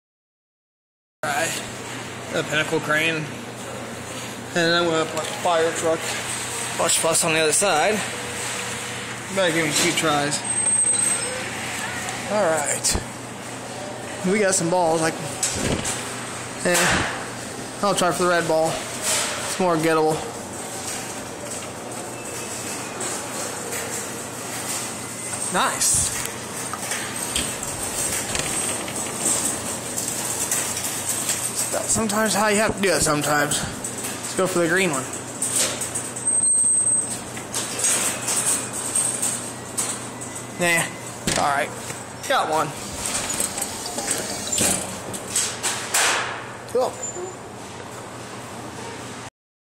PINNACLE CLAW MACHINE@ WALMART LUCKY BALL WIN
TODAY I HEAD OVER TO MY LOCAL WALMART TO FACE THE PINNACLE CLAW MACHINE HERE I AM GOIN FOR SMALL SPIKE BALLS THE CLAW IS ROUGHLY TWICE THE SIZE OF THE BALLS SO HOPE U ENJOY